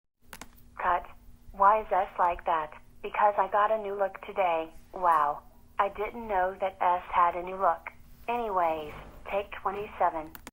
Jumpstart Logo Bloopers take 26: S' new look!
Hi guys!

I'm so excited to announce that Jumpstart Logo Bloopers is back, with some new mouths as well! This is to make producing easier, so I spend less time working on takes like these. These mouths are a new generation for all of us.

-ARTHUR